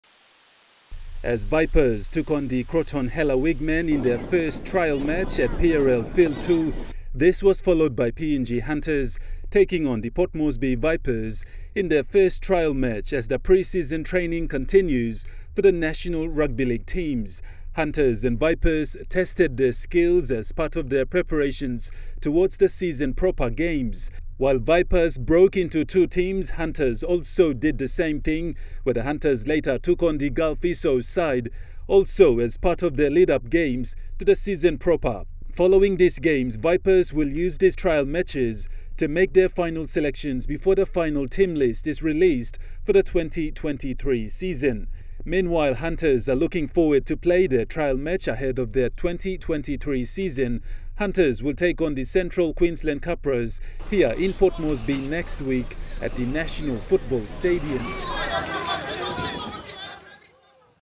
HUNTERS TRIAL MATCH
In the lead up to the season proper, today Hunters played two trial matches with both Gulf Isou and Port Moresby Vipers at PRL field 2.